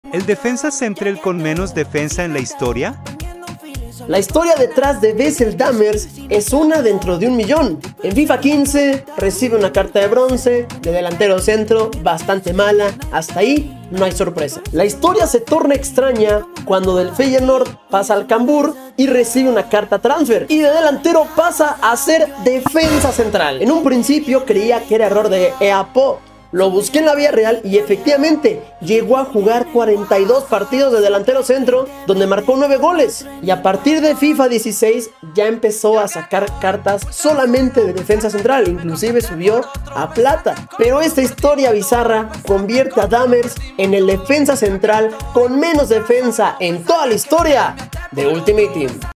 NUNCA HABIA ESCUCHADO una HISTORIA como ESTA
La increíble historia del bronce Wessel Dammers

MIS REDES:
- Directos en Twitch

- 💬 El Discord de la comunidad del canal y además ahí aviso cuando prendo directo